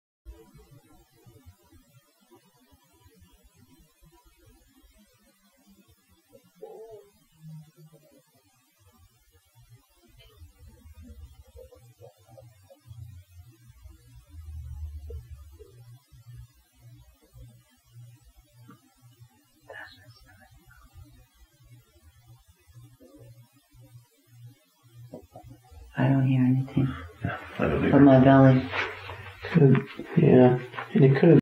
Ghost/Haunting/EVP in Anne Arundel County, Maryland ("Dozens to Let Go")
Class B EVP ("Dozens to Let Go") captured by Maryland Paranormal Research in a private residence in Arnold, Maryland on 6 Nov 2011. A disembodied voice utters the phrase... "Dozens to Let Go. " The phrase is most likely an immediate reference to an earlier word (i.e. "Taps") mentioned moments before by potentially the same disembodied voice. The phrase could also perhaps represent a veiled reference to slave trading.